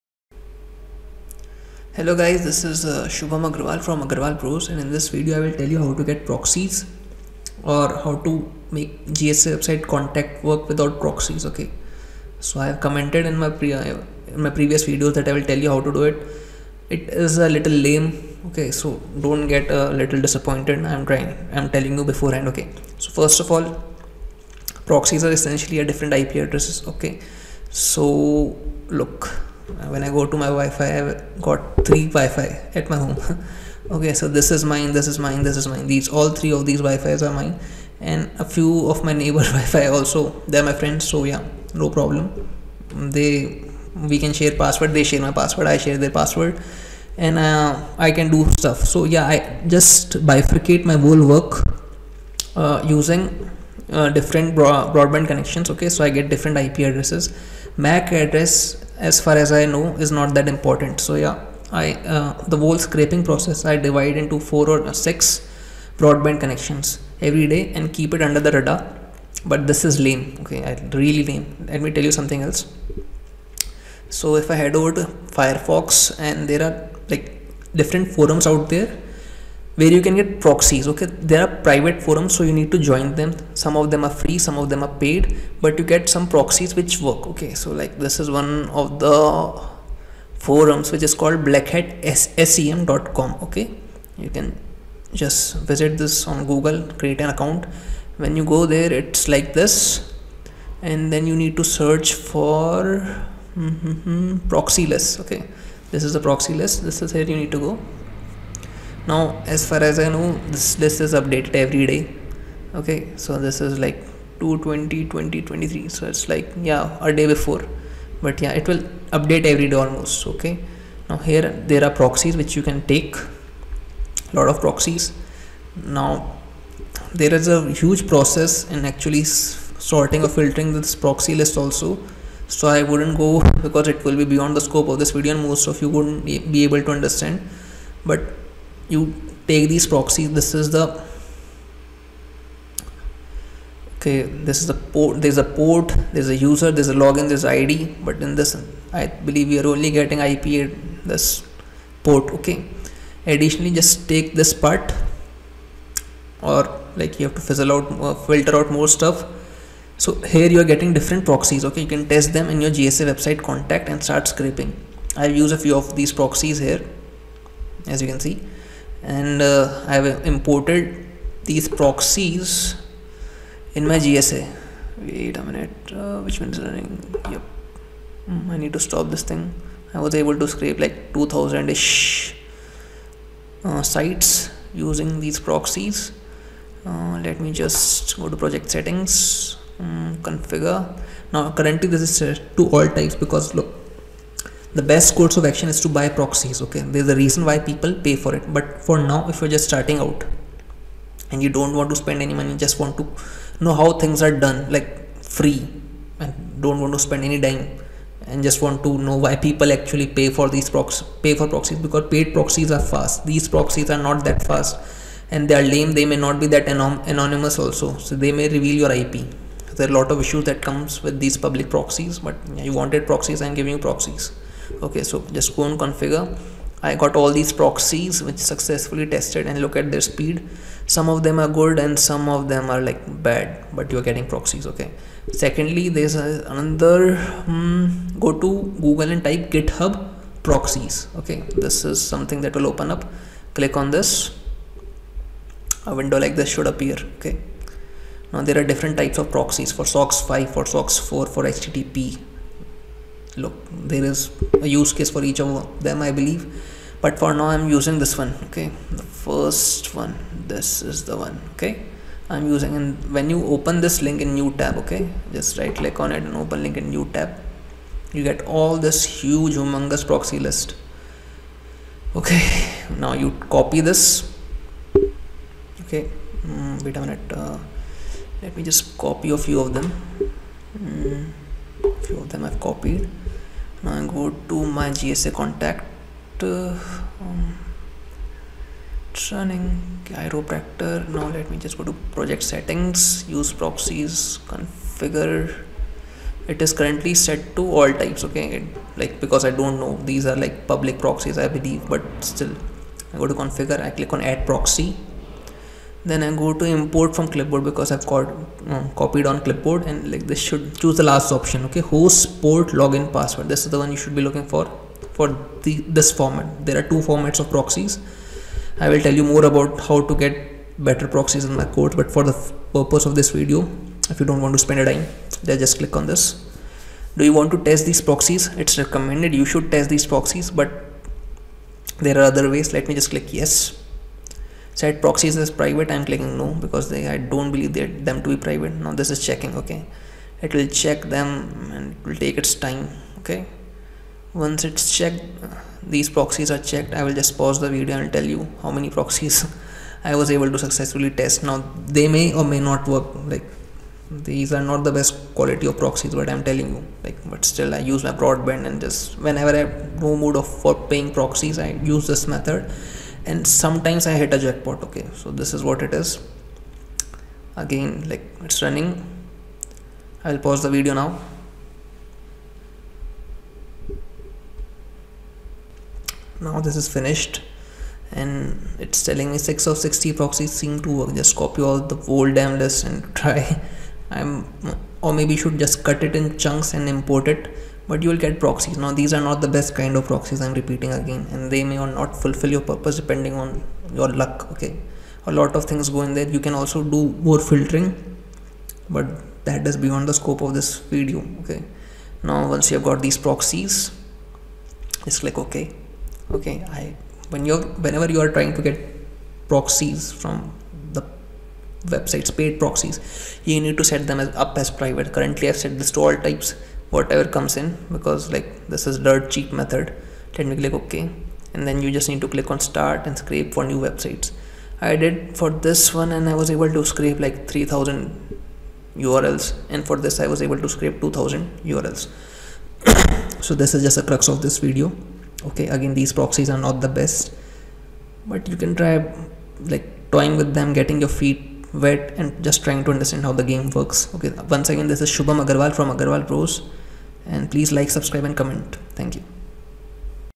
Free Proxies For GSA Website Contact
In this video, I will tell you how to use GSA Website Contact without buying proxies.